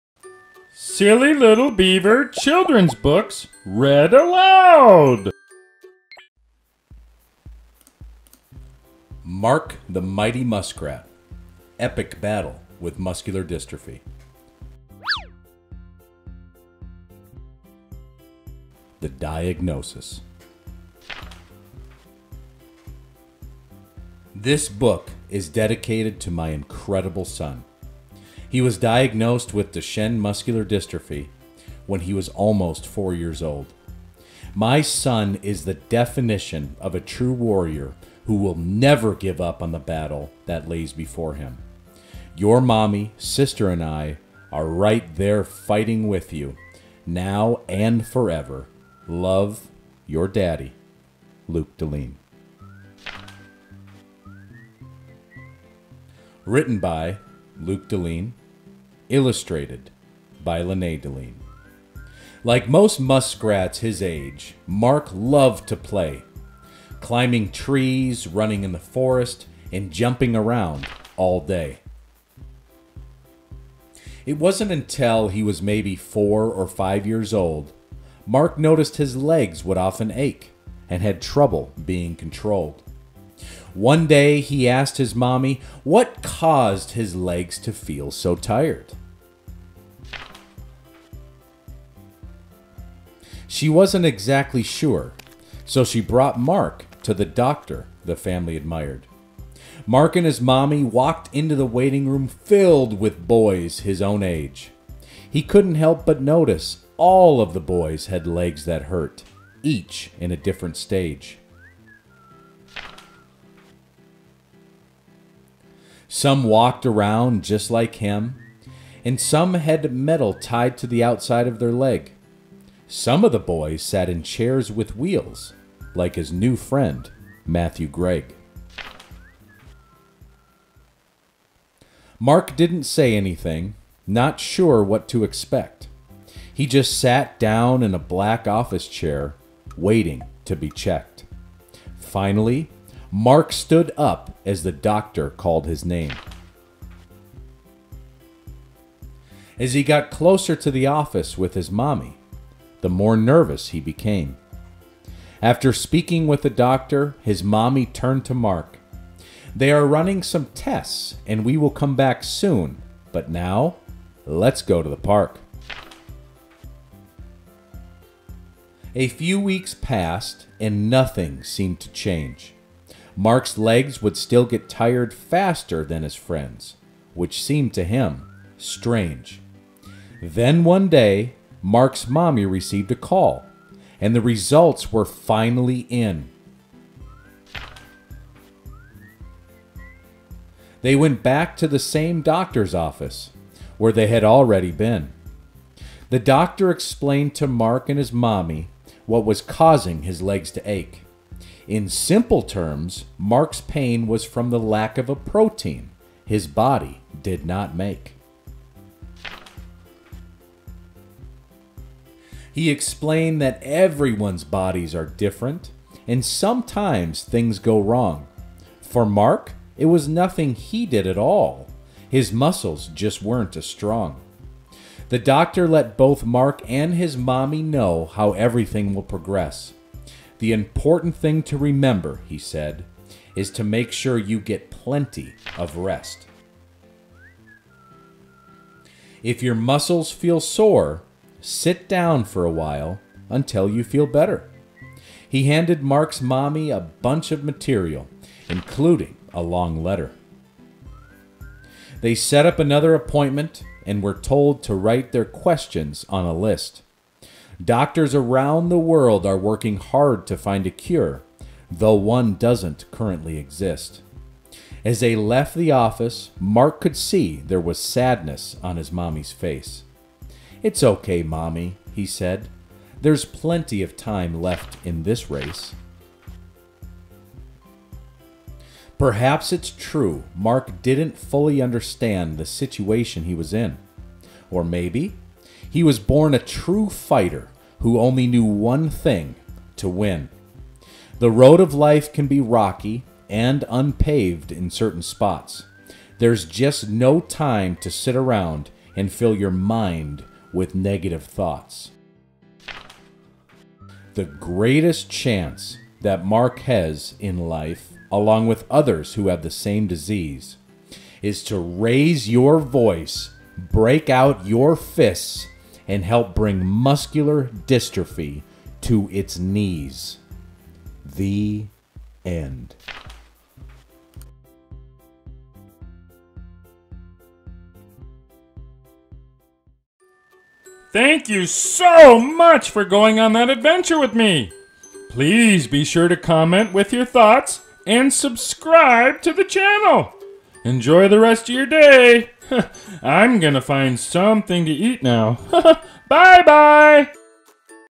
Mark The Mighty Muskrat | Epic Battle With Muscular Dystrophy | Children's Books Read Aloud
Mark The Mighty Muskrat - Epic Battle With Muscular Dystrophy is a real-life story about a family receiving a diagnosis of Duchenne Muscular Dystrophy.

This kids book is written in poetry style with fun illustrations!

It's a true story experienced by the author, Luke Dalien. His son was diagnosed with Duchenne Muscular Dystrophy, and the family struggled with how to explain the diagnosis process to their son AND his older six-year-old sister.

The truth is, this book helps children with most diagnosis understand what they're going through. From Autism to cancer and anything else, this children's book will help kids of all ages understand there's something wrong with their bodies, but the war on that disease has just begun.

Please comment with thoughts on the book or personal experiences you've had going through a diagnosis.